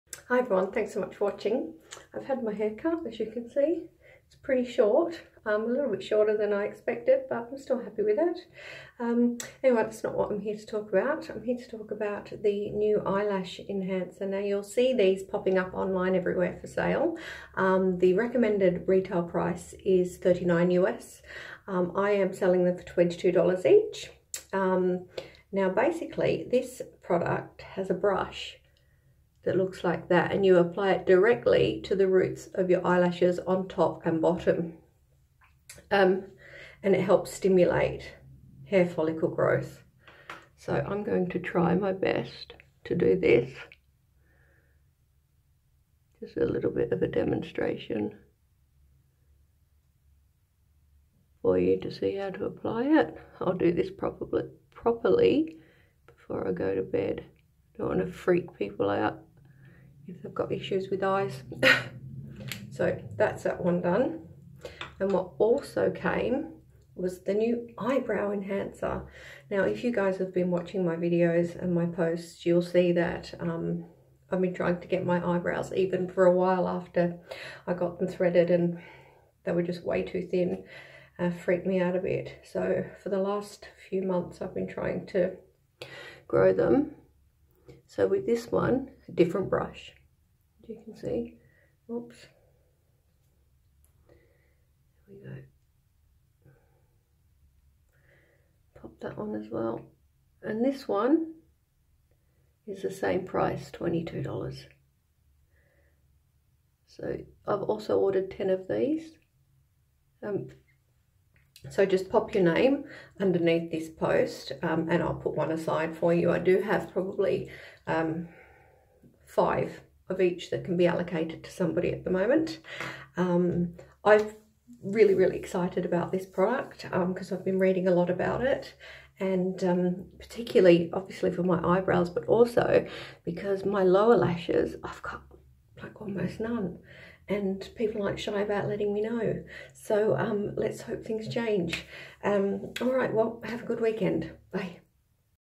FEG Eyelash and Eyebrow enhancer
Review of FEG Eyelash and Eyebrow enhancer. Week 1.